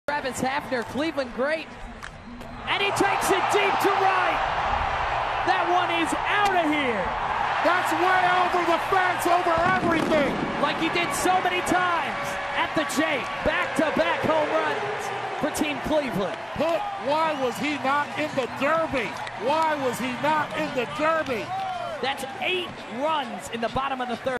Travis Hafner Hits A Real Home Run At The Celebrity Softball Game 2019
Travis Hafner hits one over the fence in a softball game